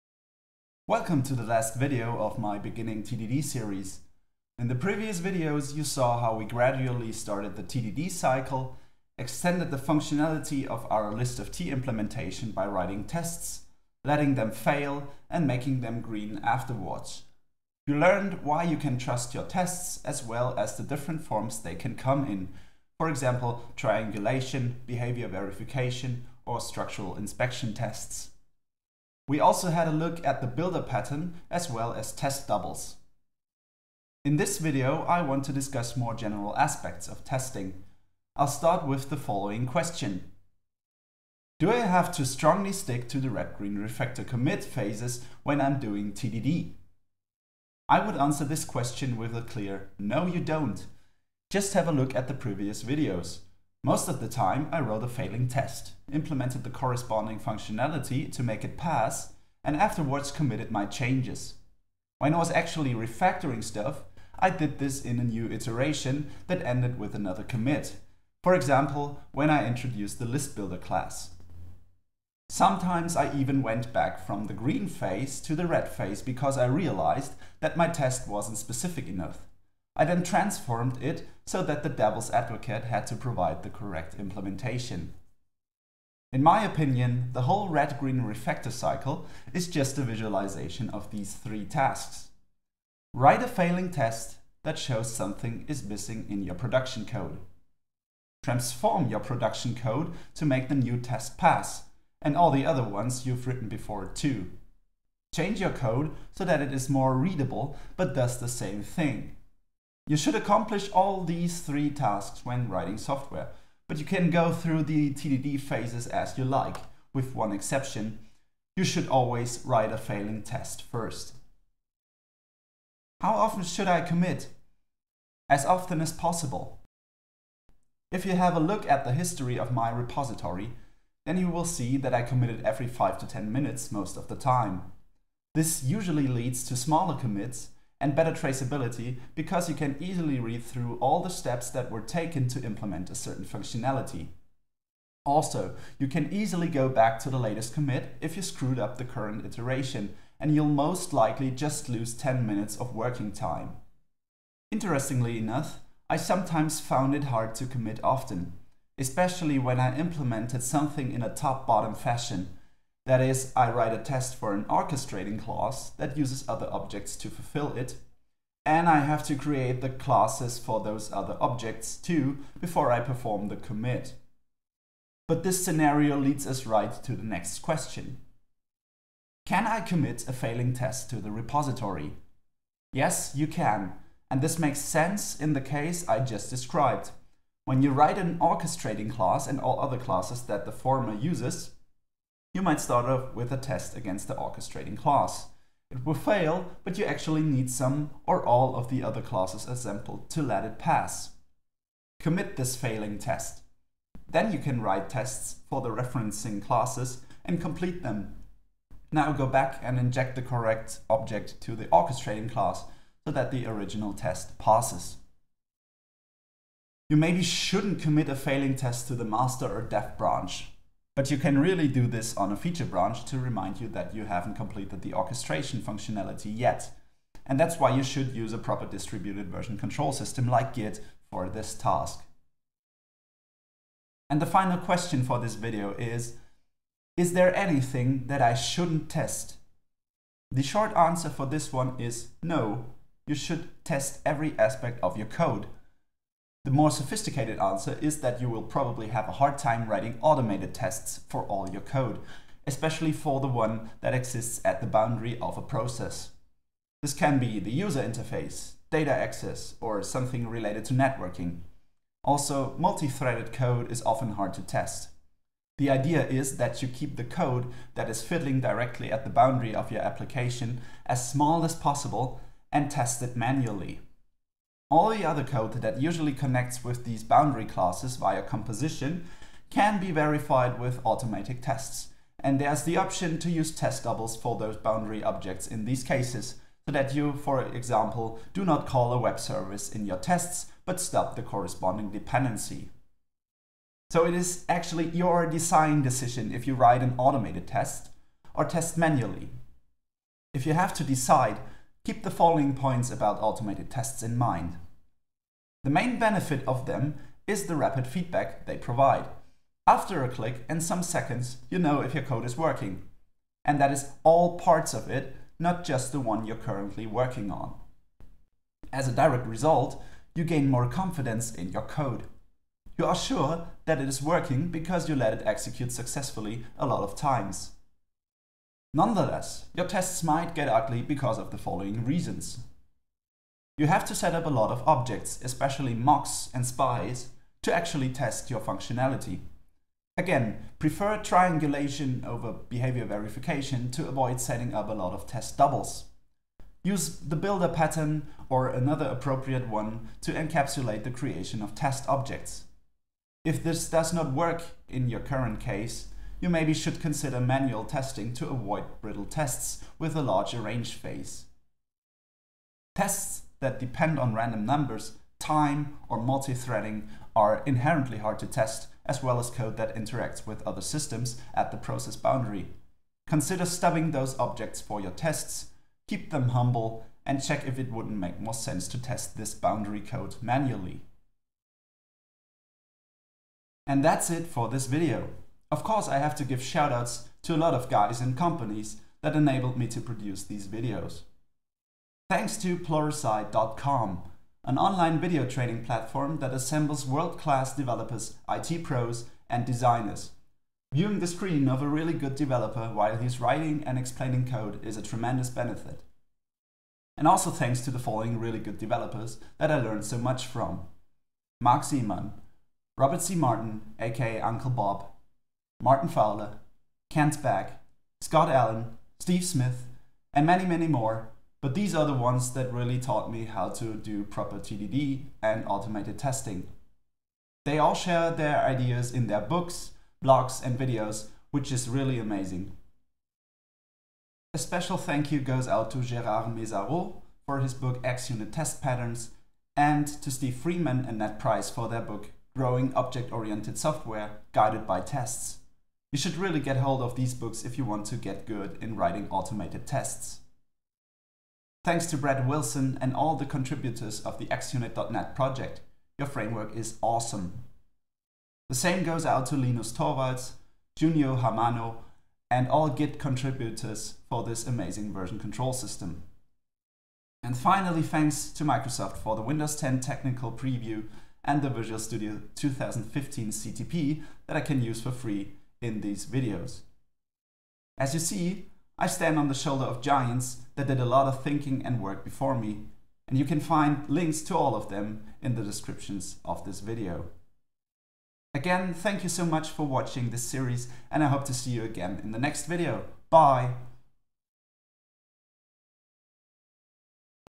Beginning TDD - General advise for automated tests
Watch this video with 1.5 or 2.0 speed and save some time!

In this video, I finish the Beginning TDD series with some general advise for automated tests.